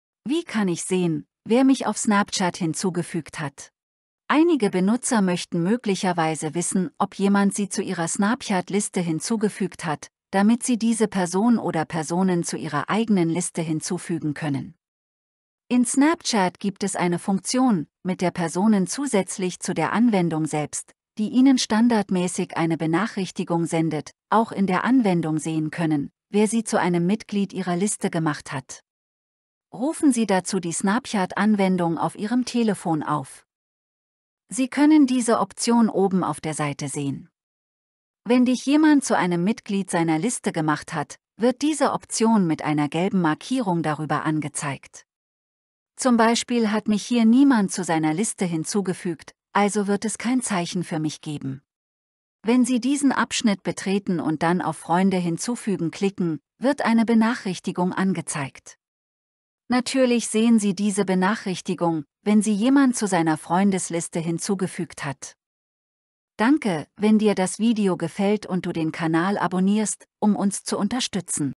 wie kann ich Sehen wer mich in Snapchat Hinzugefügt hat 2023
Ich zeige es dir in diesem Video wie kann ich Sehen wer mich in Snapchat Hinzugefügt hat.